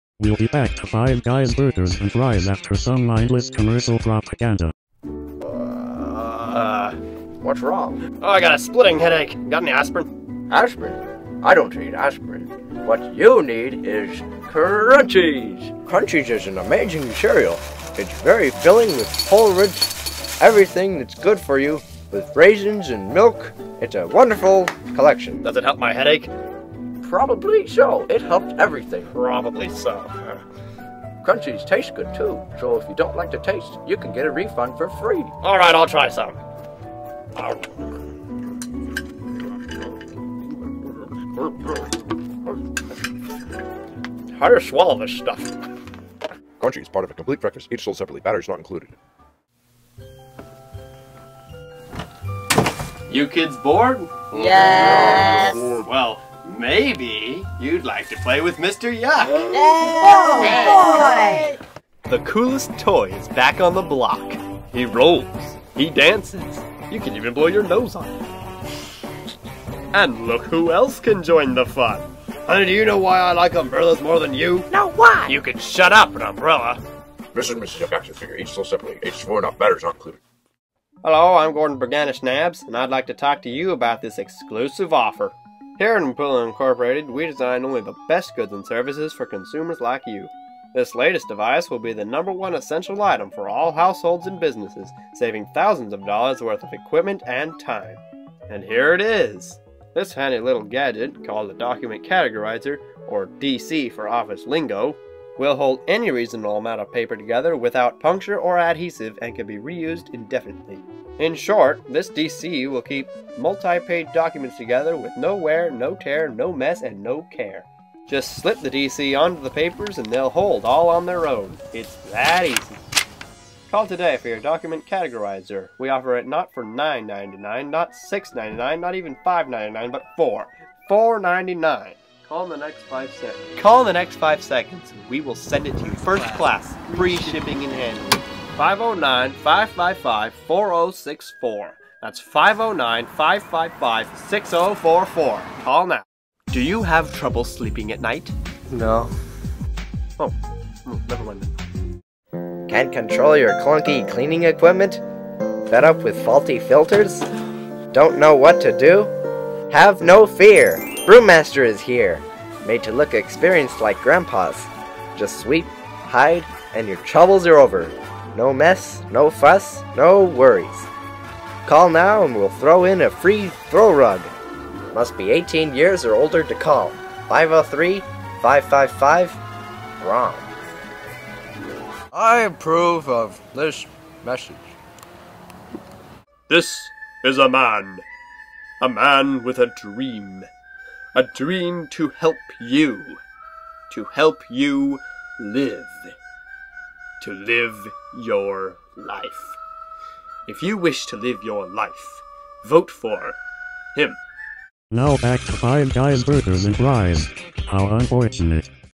Tuesday Commercials (5D5D re-up)
Slew of commercials based on the ones I grew up watching on cable. Originally uploaded for Advertisements Week, June 21st, 2011 to Five Dudes Five Days.